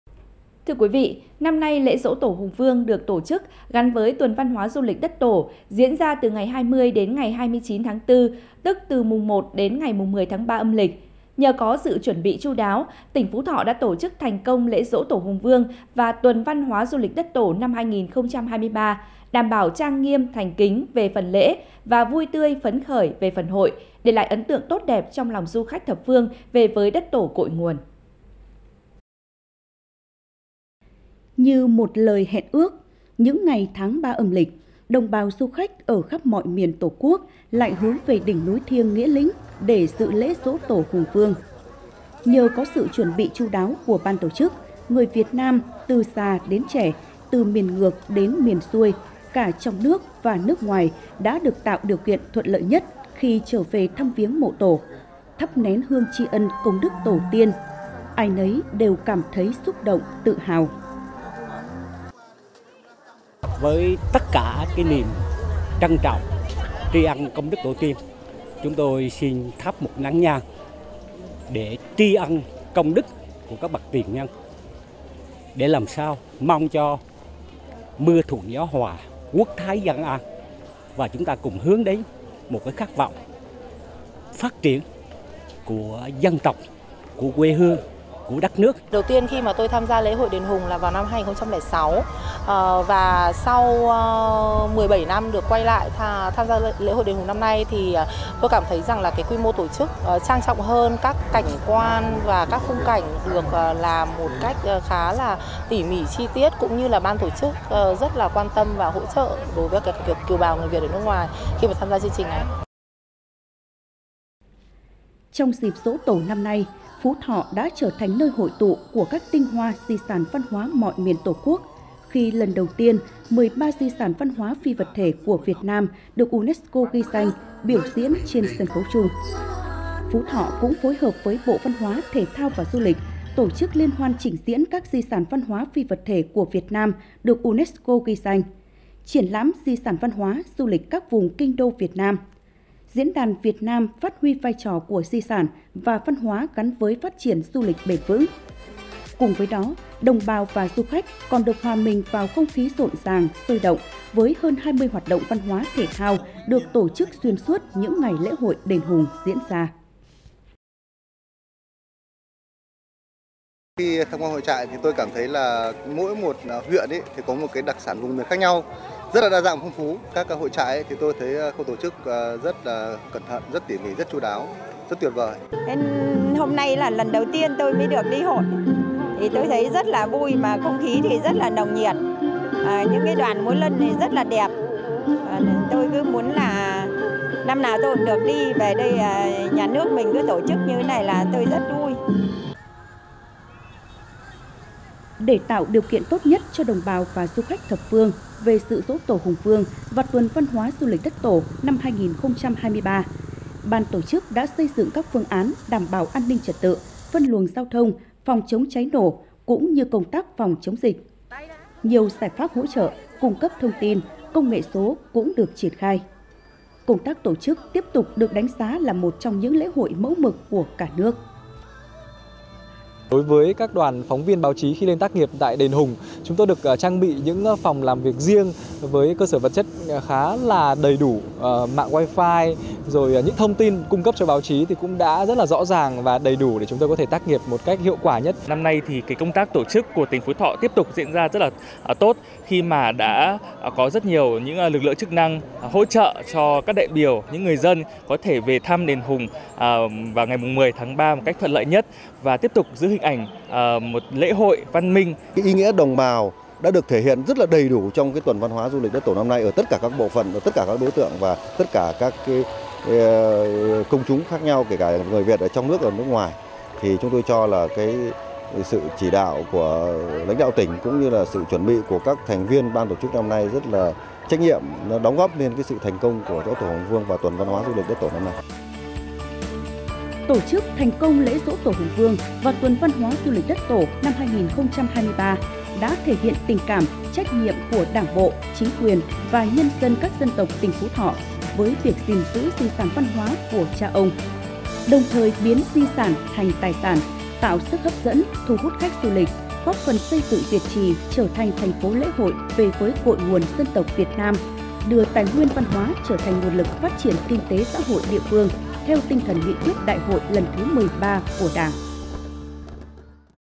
Tổ chức thành công Lễ Giỗ Tổ Hùng Vương - Tuần văn hóa du lịch đất Tổ năm 2023
Nhờ có sự chuẩn bị chu đáo, tỉnh Phú Thọ đã tổ chức thành công lễ Giỗ Tổ Hùng Vương và Tuần Văn hóa - du lịch đất Tổ năm 2023, đảm bảo trang nghiêm, thành kính về phần lễ và vui tươi, phấn khởi về phần hội, để lại ấn tượng tốt đẹp trong lòng du khách thập phương về với đất Tổ cội nguồn.